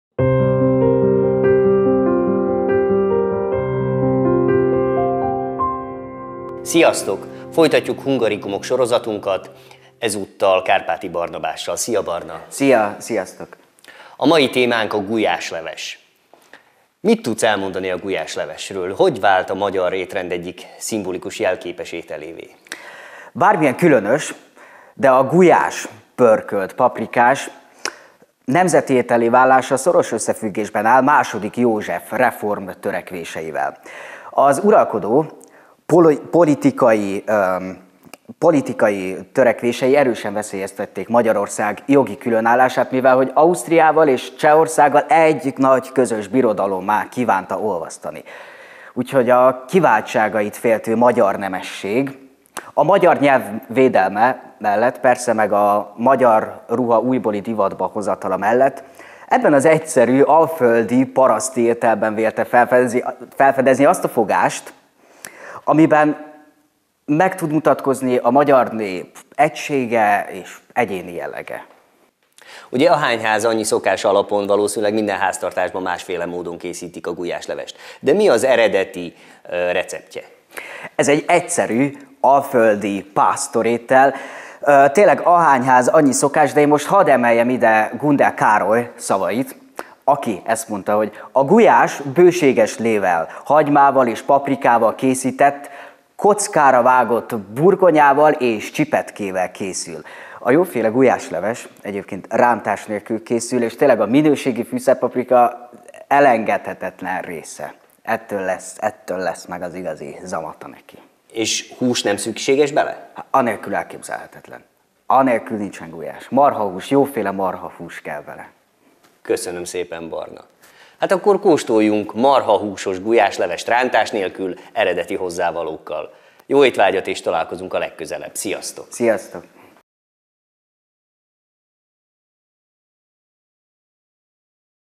Online Erzsébet-játékok – Déryné program: Hungarikumok - Gulyásleves
A Hungarikumon Gyűjteményében a magyarság történelmi és jelenkori felfedezései, újításai, valamint természeti kincsei egyaránt fellelhetőek. A gyűjtemény évről évre bővül, hiszen Magyarország bővelkedik a csodálatosabbnál csodálatosabb értékekben. Ezeket az értékeket mutatja be a Déryné program társulata.
A magyarok jelképes ételéről, a gulyásleves történetéről és számos érdekességről Havasi Péter, Kárpáti Barnabással beszélget.